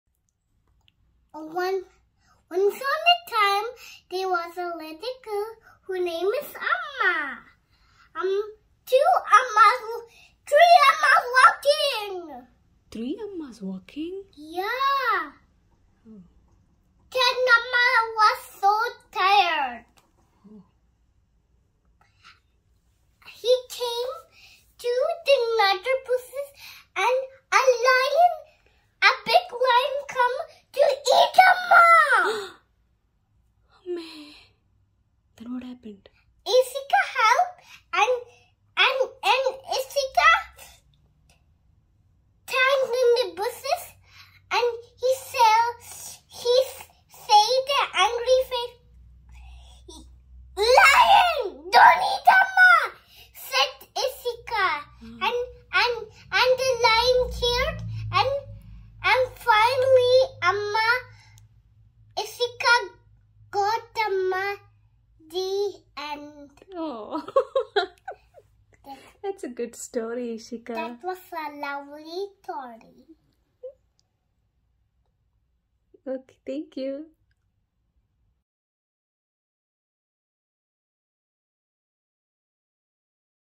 She made up a story. Scary and Spooky story telling by a 3 year old.
I could capture this story telling of hers. Watch till the end.